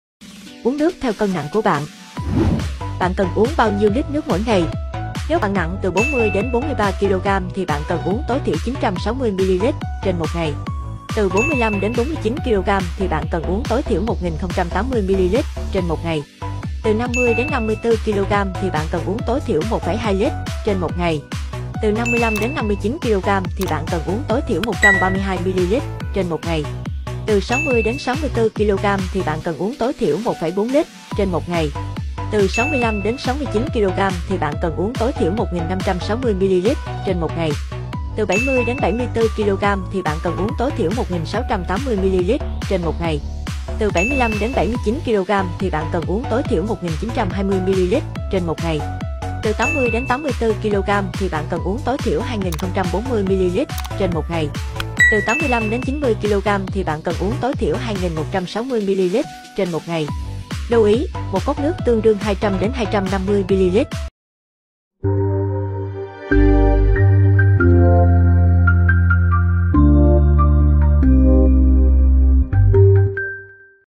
Bạn cần uống BAO NHIÊU lít nước / 1 ngày ? - Kênh của Lúa
Bạn có biết cơ thể mình cần uống bao nhiêu ml nước 1 ngày không? Hãy cùng xem Video này để hiểu thêm nhé!